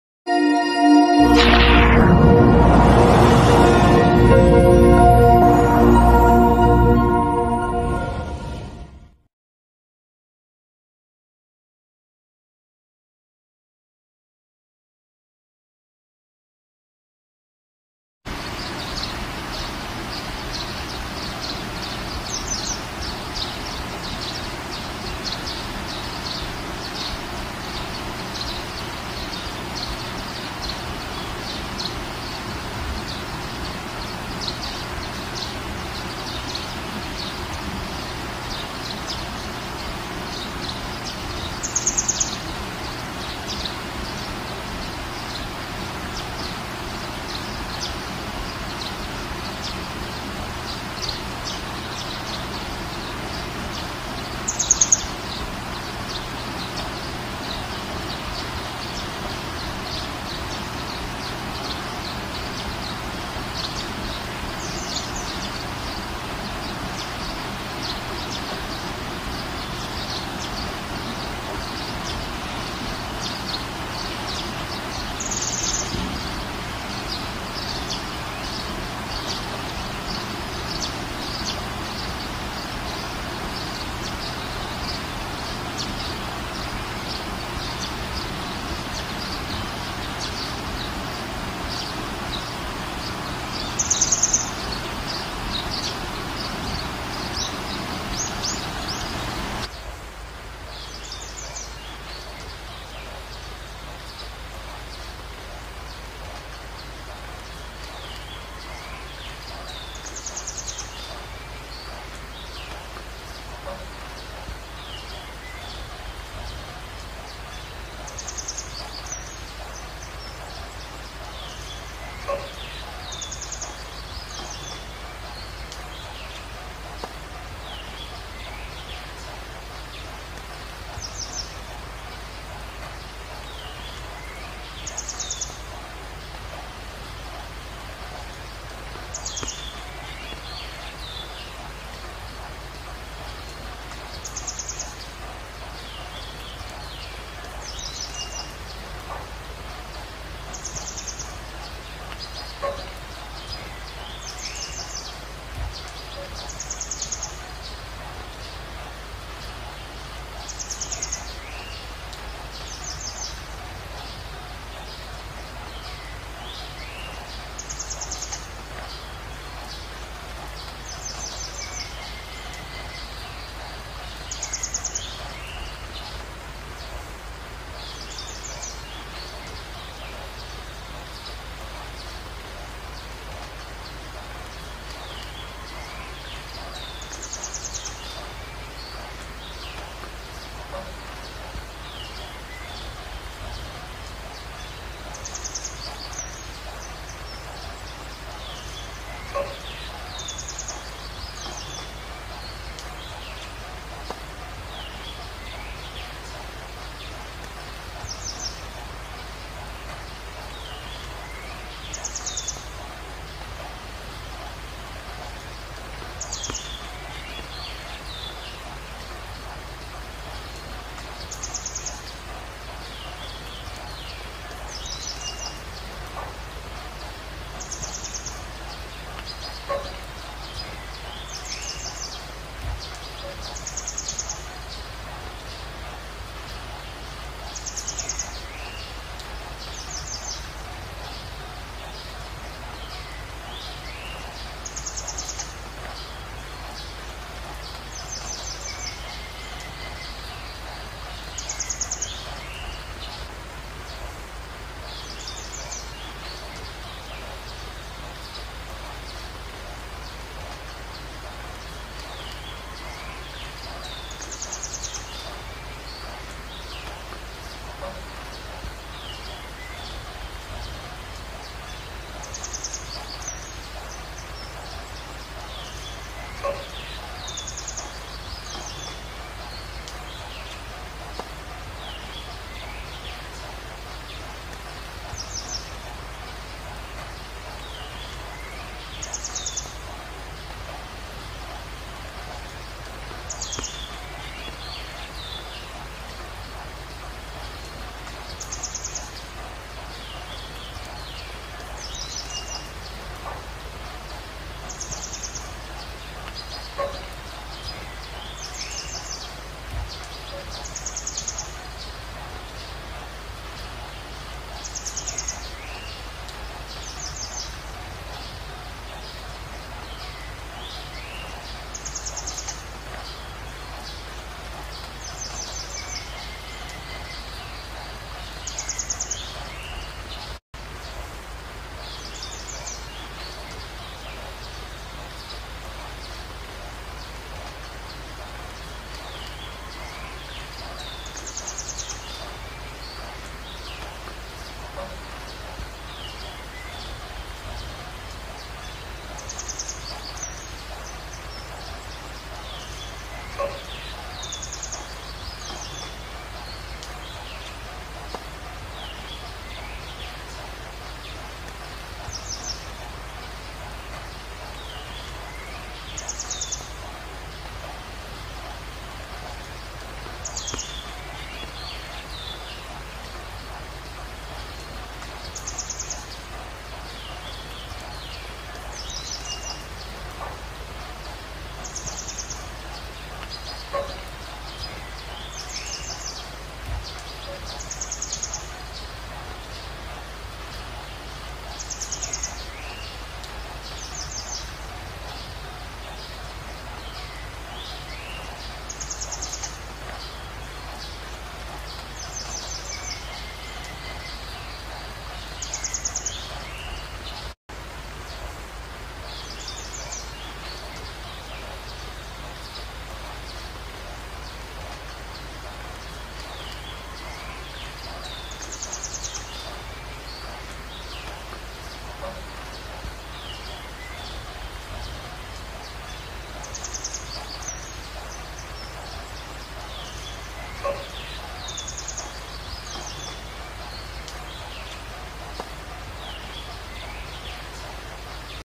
Cara Membuat Bonsai Kelapa Gantung Terbalik
Berbagai cara tanam bonsai kelapa dilakukan untuk memberikan sentuhan estetika. Salah satunya dengan cara yang ada divideo ini.